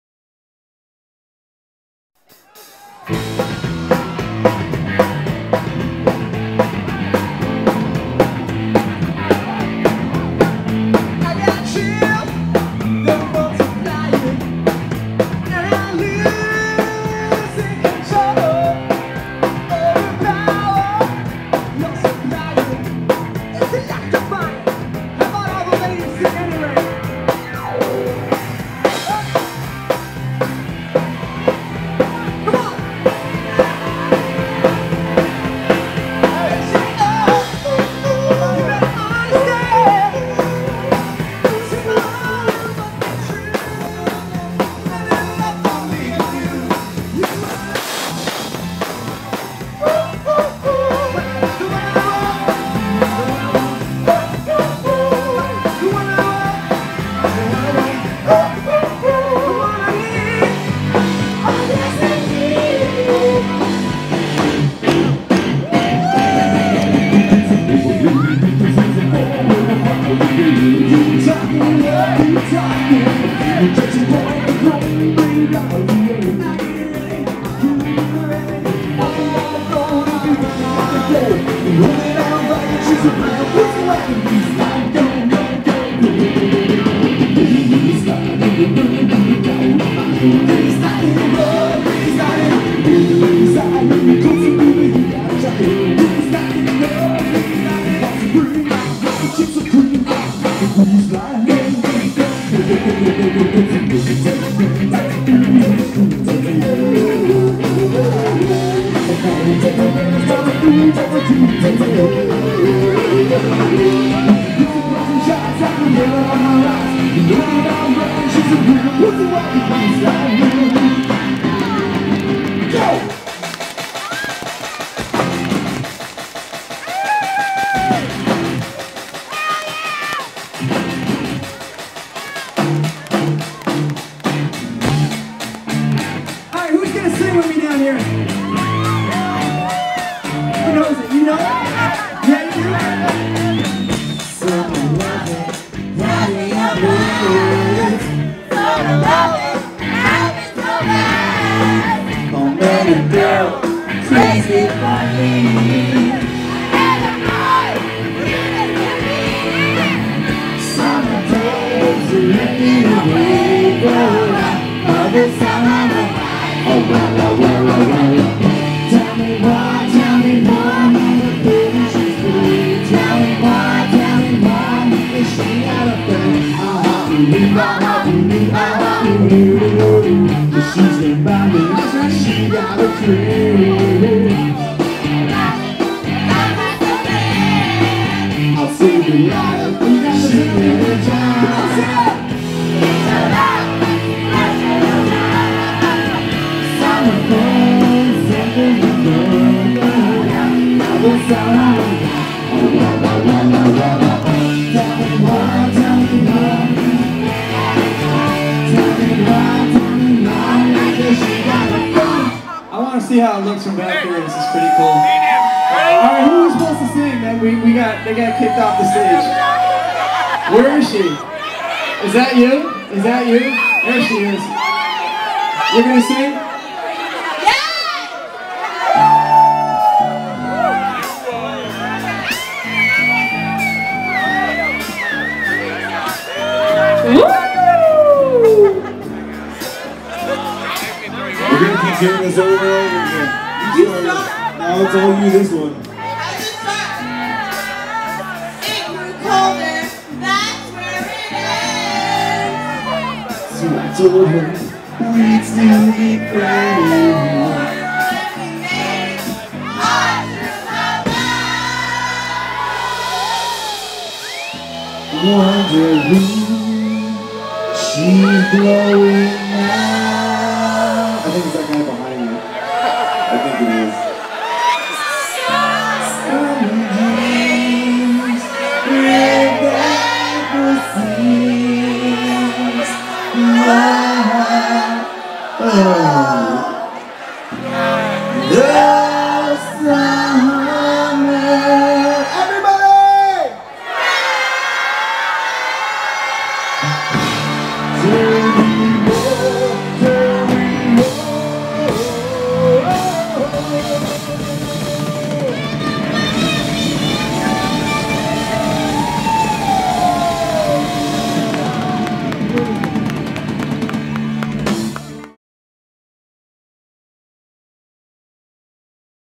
Nuts at Alive at Five
Nuts In A Blender at Alive @ Five in Albany, NY July 23, 2009